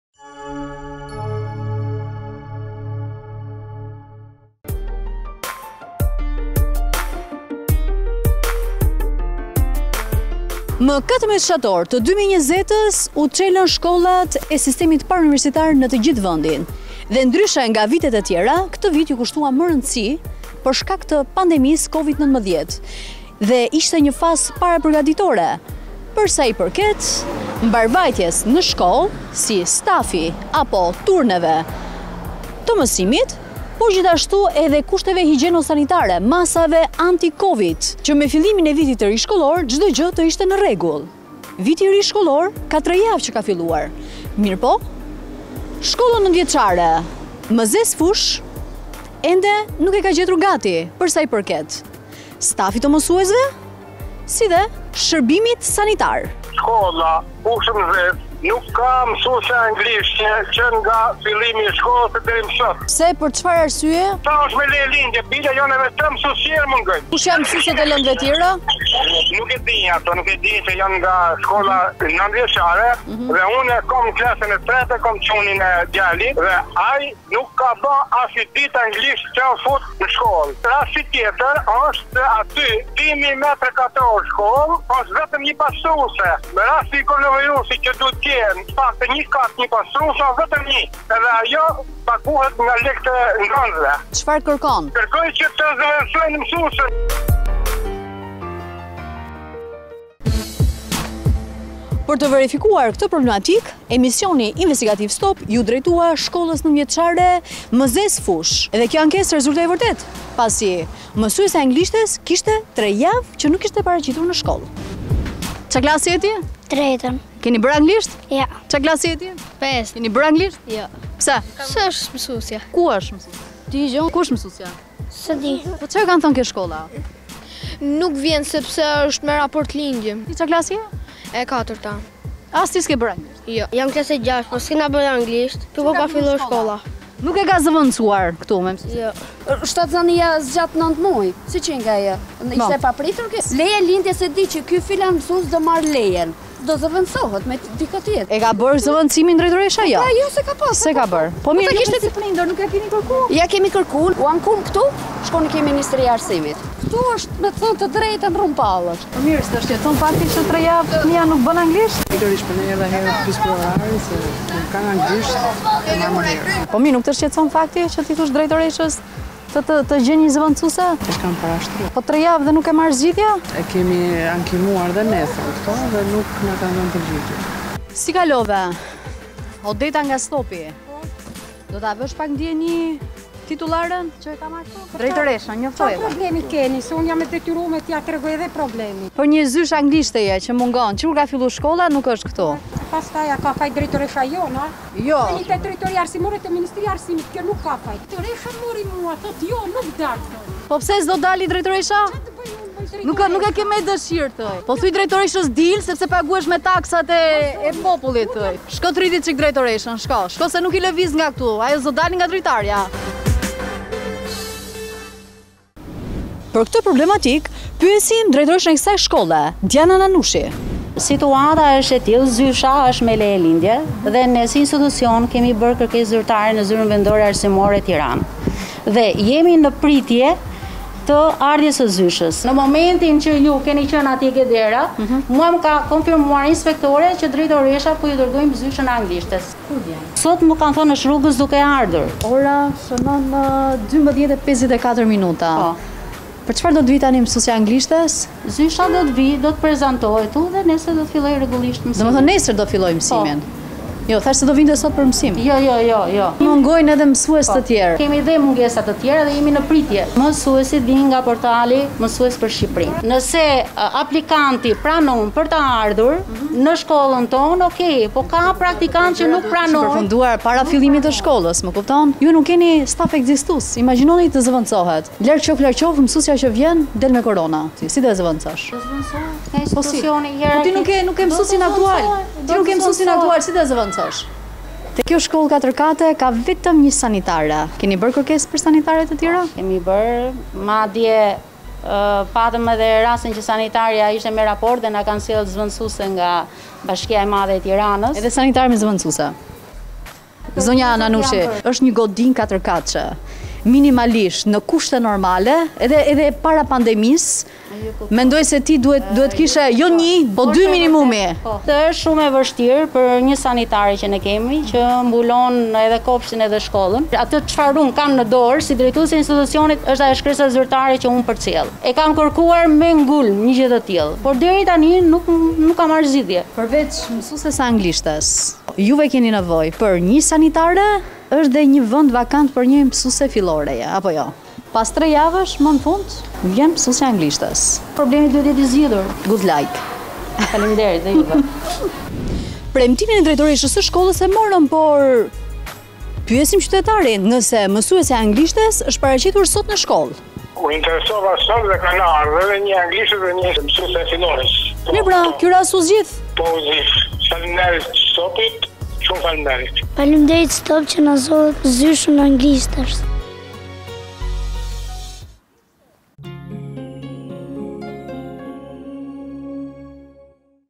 Tre jave pas nisjes se shkolles ,shkolla pa mesuese anglishteje! - Stop - 12 Tetor 2020
Në 14 shtator të 2020-ës, nisi viti shkollor në sistemin parauniversitar në vend. Por “Shkolla 9 vjecare Mëzez- Fushë”, ende nuk është gati, përsa i përket stafit të mësuesve, si dhe shërbimit sanitar.” Prindër dhe nxënës tregojnë, se që kur ka nisur shkolla, nuk kanë bërë asnjë orë anglisht, pasi mësuesja ka marrë lejen e lindjes dhe nuk është zëvendësuar.Përvec saj, në këtë shkollë mungojnë edhe mësues të tjerë ka vetëm një sanitare në kohë pandemie.Për këtë problematikë pyesim drejtoreshën e shkollës Diana Nanushi.Ajo thotë, se i ka nisur shkresë ZVA-së, por nuk ka marrë zgjidhje.Por drejtoresha tregon, se kur gazetarja ishte në derën e shkollës, e telefonuan nga ZVA-ja, për t’i komunikuar, se mësuesja e anglishtes ishte rrugës...!!!